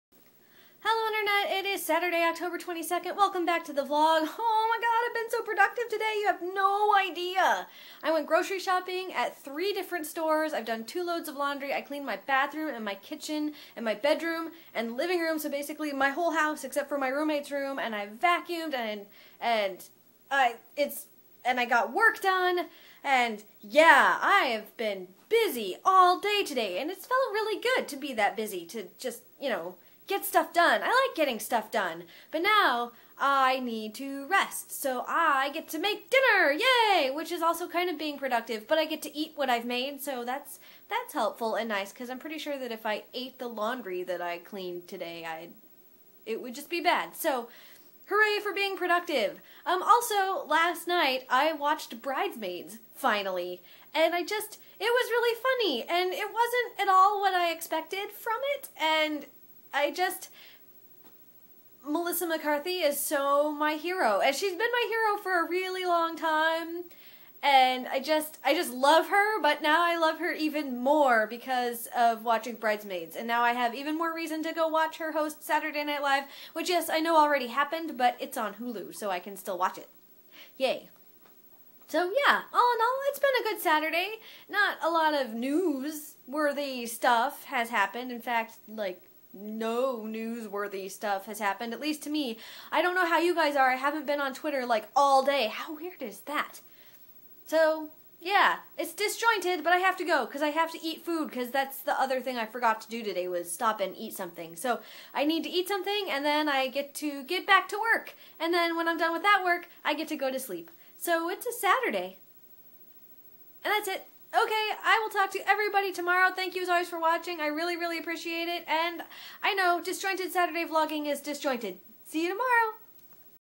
Disjointed. But Productive! (October 22, 2011)
I got so! Much! Done! Today! And now? I must eat (so I have the stamina to get more work done).